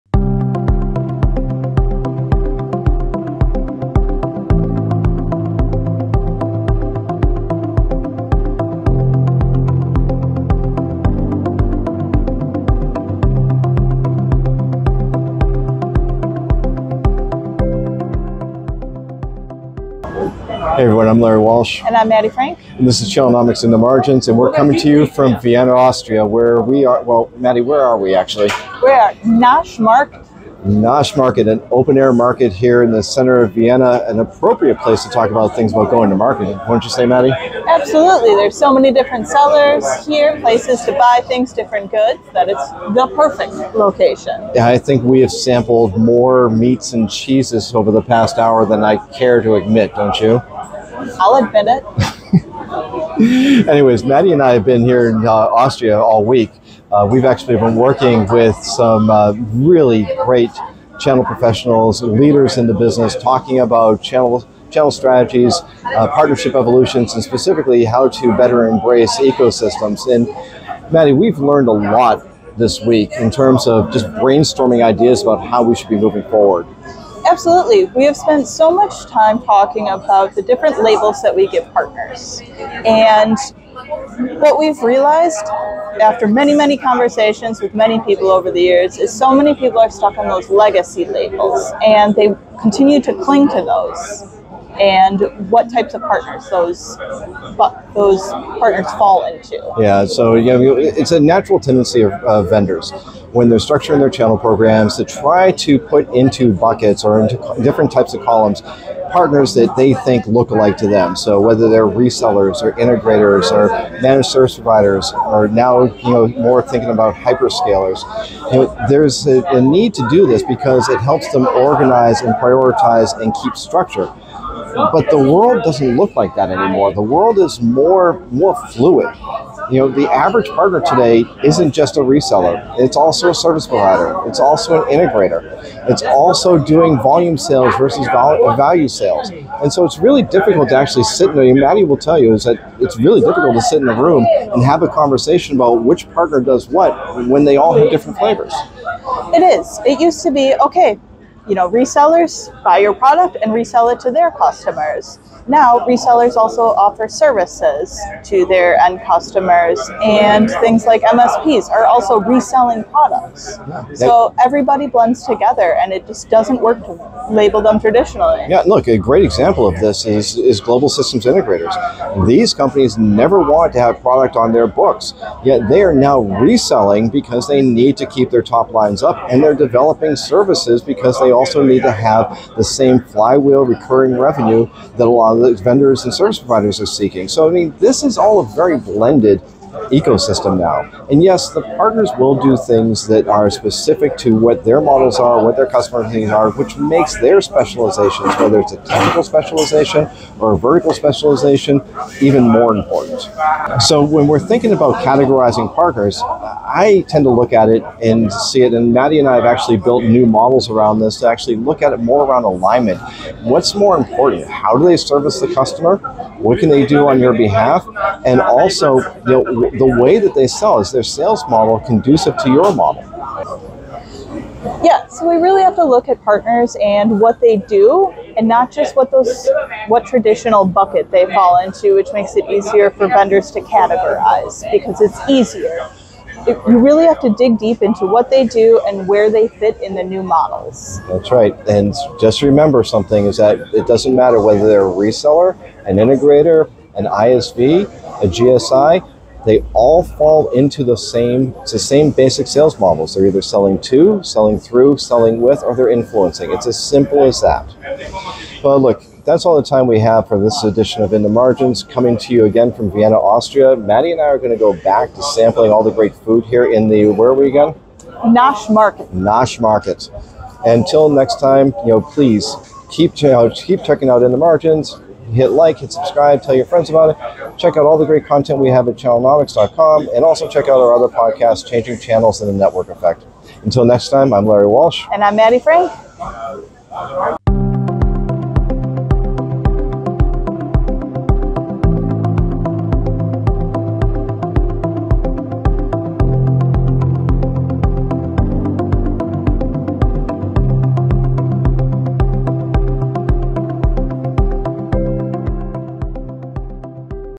In Vienna, Rethinking What We Call Partners
Partners operating in the channel have evolved over the years. They are far from monolithic and cannot be easily labeled. However, vendors and channel leaders persist in categorizing partners into traditional, homogeneous groups such as resellers, integrators, managed service providers, and so on. During a channel leadership retreat in Vienna, Austria, Channelnomics's Larry Walsh and Maddie Frank collaborated with a group of channel leaders to discuss the evolution of indirect routes to market and ecosystems. A key takeaway from these discussions was the recognition that it's time for the channel to move beyond simplistic labels and instead focus on partners' alignment with go-to-market models.